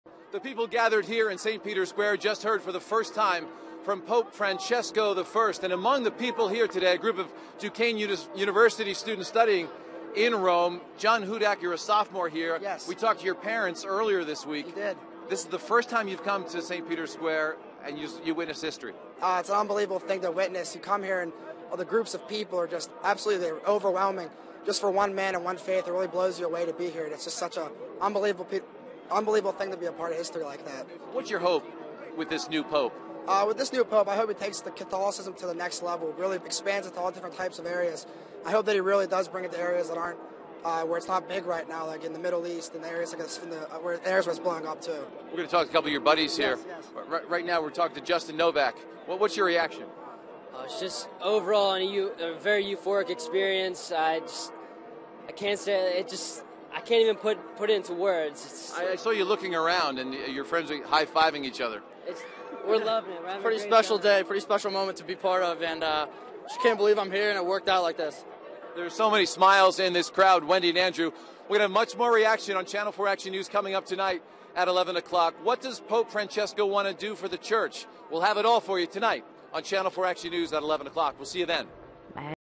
Mike Live from Rome: Duquesne Students Witness History
Choosing the Pope: Action News' Mike Clark is live from Rome with Duquesne Students such as John Hudock who watched first hand history being made with Pope Francis.   Complete Coverage